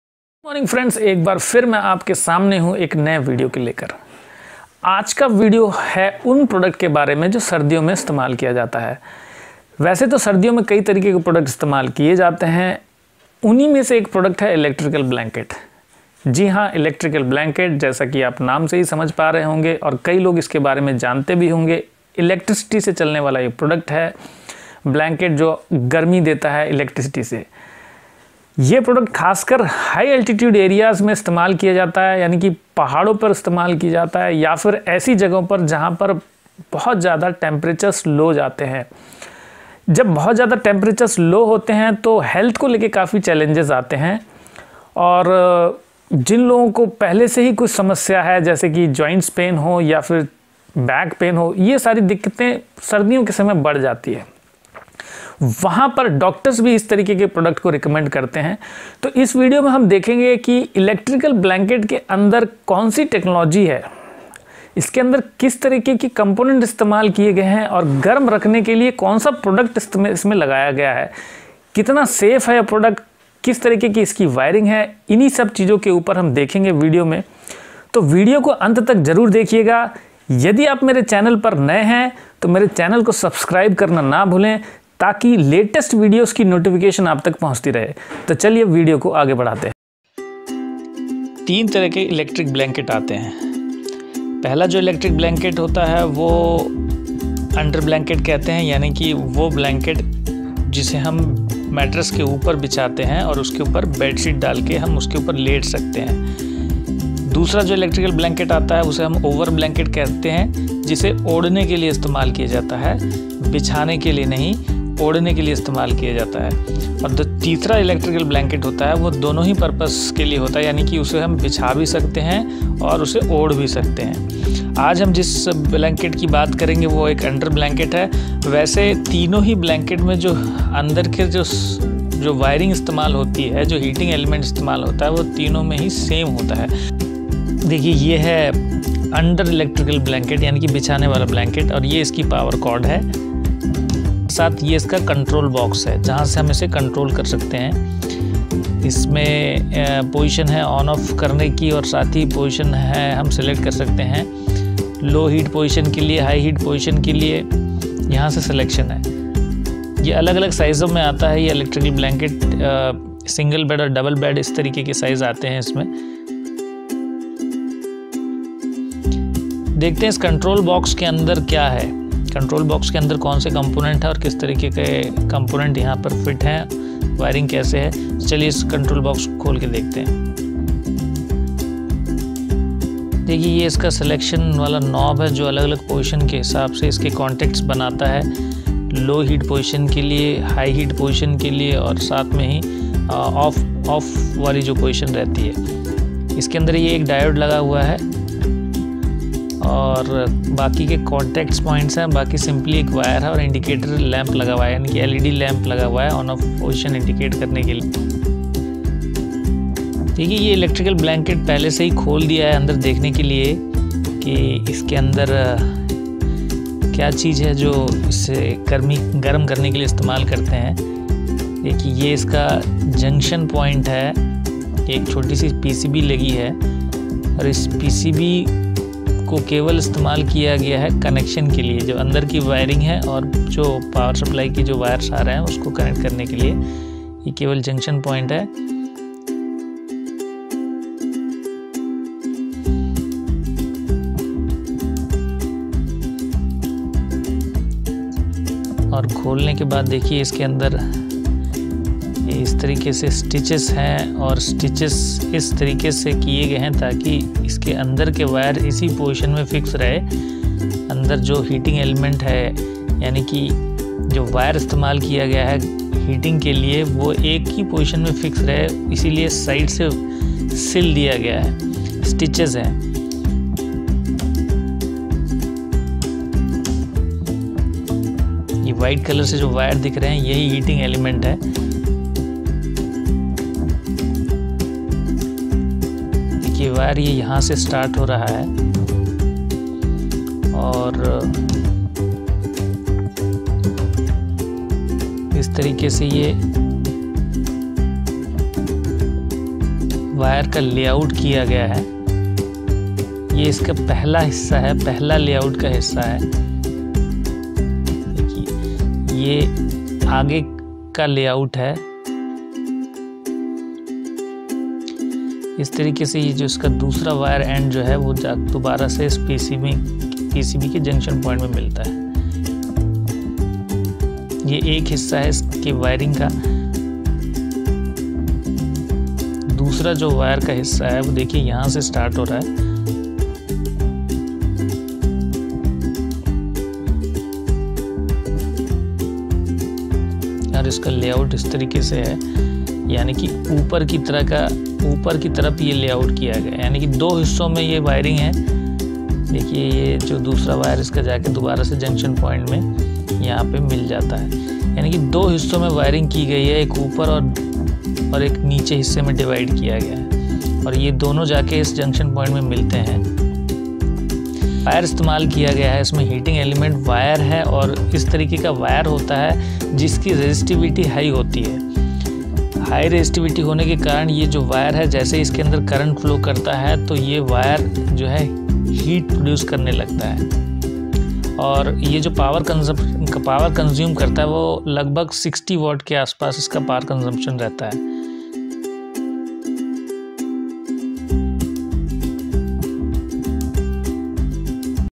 HOW TO REPAIR ELECTRIC BLANKET// HOW IT WORKS, WIRING LAYOUT AND COMPONENT AND PRECAUTION
ELECTRIC BLANKET: HOW IT WORKS, REPAIRING OF ELECTRIC BLANKET, WIRING AND COMPONENT, ELECTRIC BED WARMER

 What you will get in this video:
1. What is electric is Electric Blanket.
2. Types of electric blanket.
3. Component used in electric blanket.
4. Wiring layout explanation.
5. Power consumption of electric blanket.

Follow my on my Facebook ID : Sunil kumar